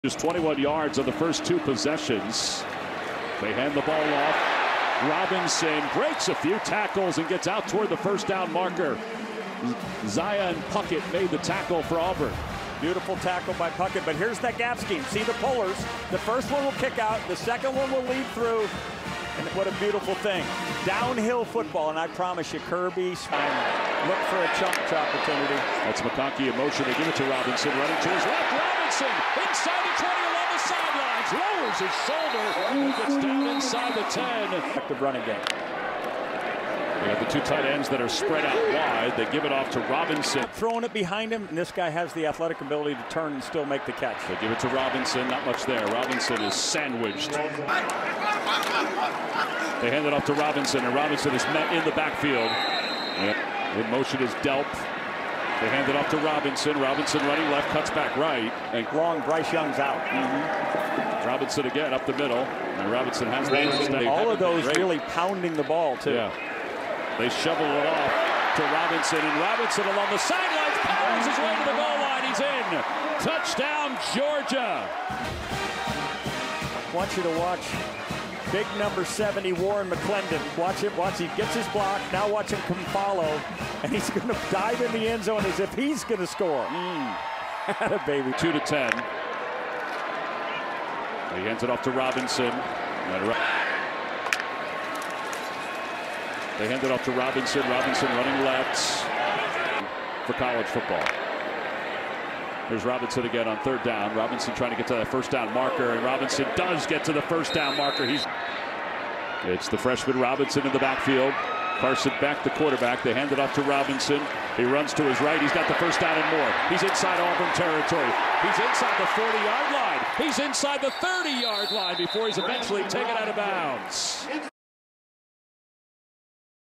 Branson Robinson Georgia RB vs Auburn 2022
Branson Robinson Georgia RB Highlights vs Auburn 2022

Scouting Tomorrow's Fantasy Players Today

Host of the Devy To Deep Dynasty Football Podcast.